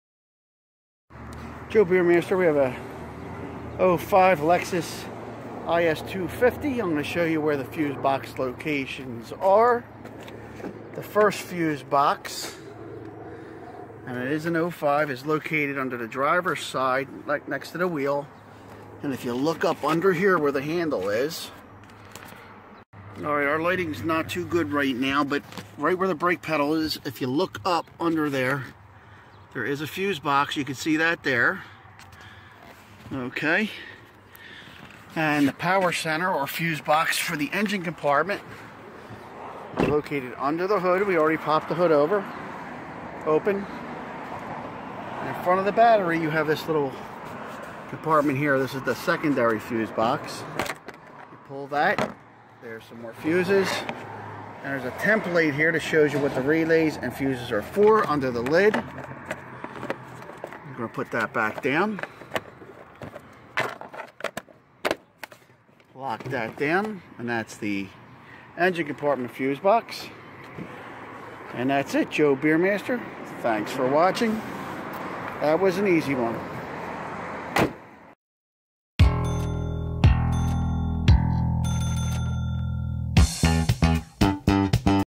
Lexus IS 250 Fuse Box Locations
Fuse box locations on Lexus IS 250 models and IS models.